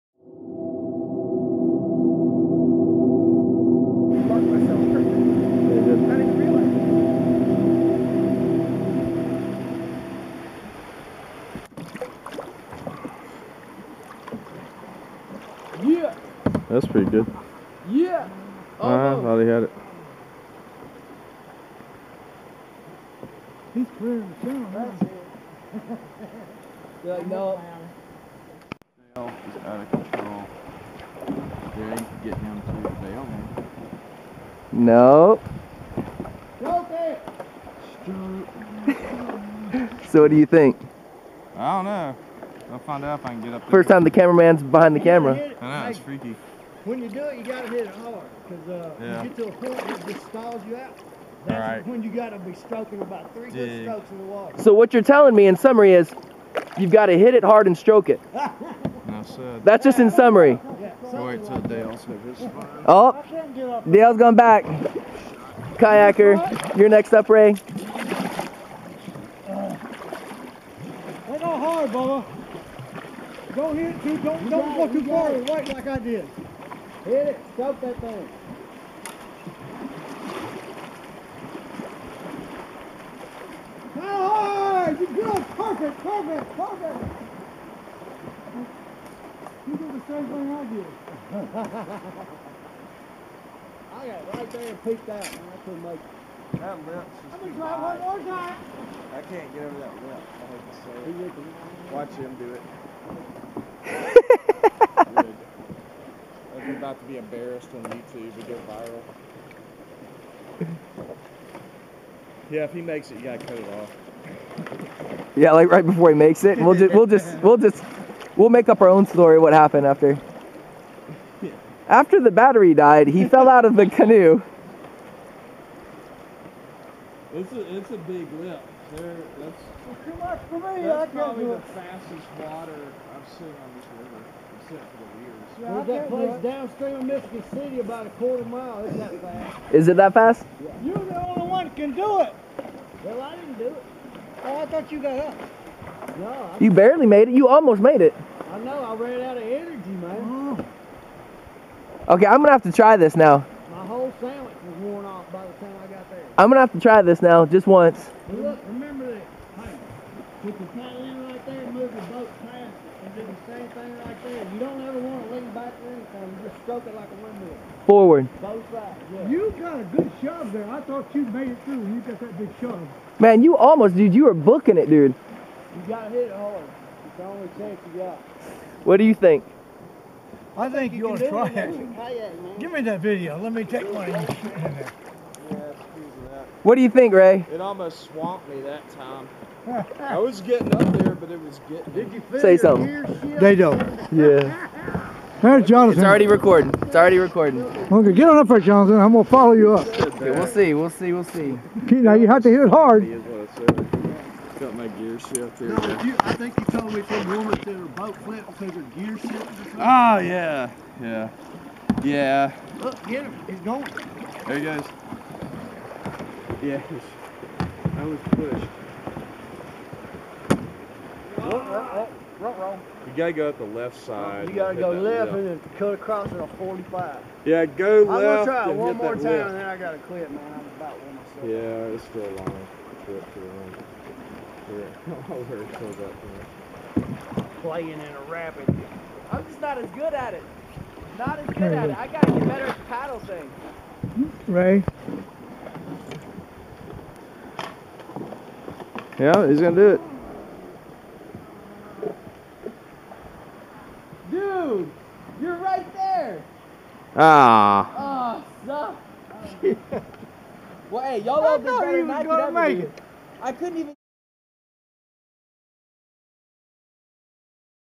River Guides Challenge the Beaver Tail Rapids
River guides attempt to paddle up a small rapid of the HWY72 to Michigan City section of the Wolf River. The rapid was created by a submerged beaver dam.